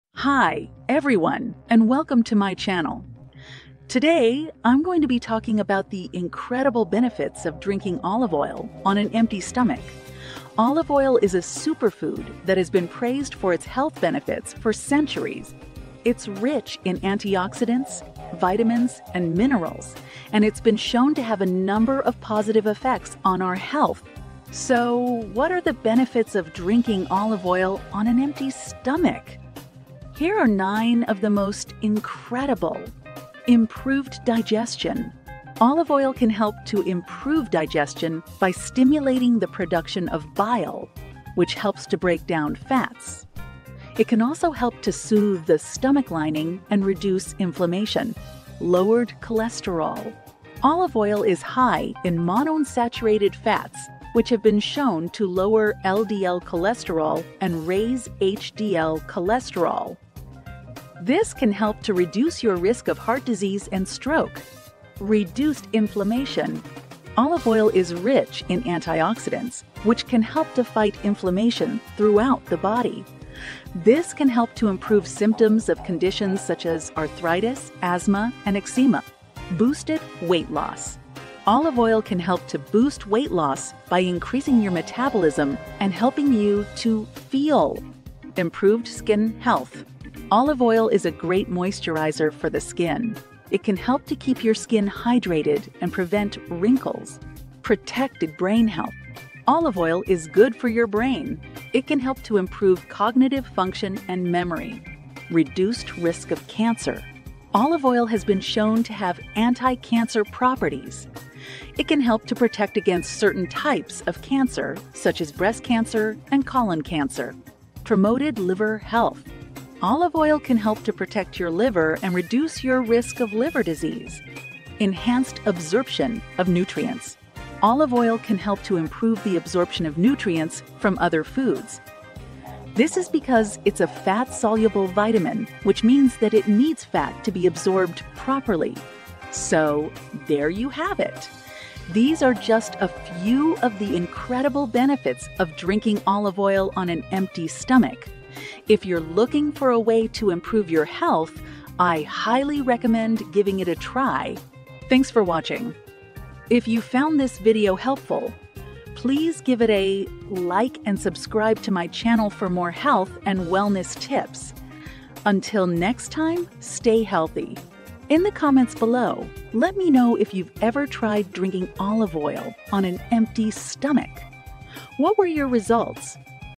Drink Olive Oil on Empty Stomach and After Days These 9 Incredible Benefits will Happen to Your Body
Today, I'm going to be talking about the incredible benefits of drinking olive oil on an empty stomach. Olive oil is a superfood that has been praised for its health benefits for centuries. It's rich in antioxidants, vitamins, and minerals, and it's been shown to have a number of positive effects on our health.

So, what are the benefits of drinking olive oil on an empty stomach? Here are nine of the most incredible:

Improved digestion:

 Olive oil can help to improve digestion by stimulating the production of bile, which helps to break down fats. It can also help to soothe the stomach lining and reduce inflammation.

Lowered cholesterol:

 Olive oil is high in monounsaturated fats, which have been shown to lower LDL (bad) cholesterol and raise HDL (good) cholesterol. This can help to reduce your risk of heart disease and stroke.

Reduced inflammation:

 Olive oil is rich in antioxidants, which can help to fight inflammation throughout the body. This can help to improve symptoms of conditions such as arthritis, asthma, and eczema.

Boosted weight loss:

 Olive oil can help to boost weight loss by increasing your metabolism and helping you to feel full.

Improved skin health:

 Olive oil is a great moisturizer for the skin. It can help to keep your skin hydrated and prevent wrinkles.

Protected brain health:

 Olive oil is good for your brain. It can help to improve cognitive function and memory.

Reduced risk of cancer:

 Olive oil has been shown to have anti-cancer properties. It can help to protect against certain types of cancer, such as breast cancer and colon cancer.

Promoted liver health:

Olive oil can help to protect your liver and reduce your risk of liver disease.

Enhanced absorption of nutrients:

Olive oil can help to improve the absorption of nutrients from other foods. This is because it's a fat-soluble vitamin, which means that it needs fat to be absorbed properly.

So, there you have it. These are just a few of the incredible benefits of drinking olive oil on an empty stomach. If you're looking for a way to improve your health, I highly recommend giving it a try.

In the comments below, let me know if you've ever tried drinking olive oil on an empty stomach. What were your results?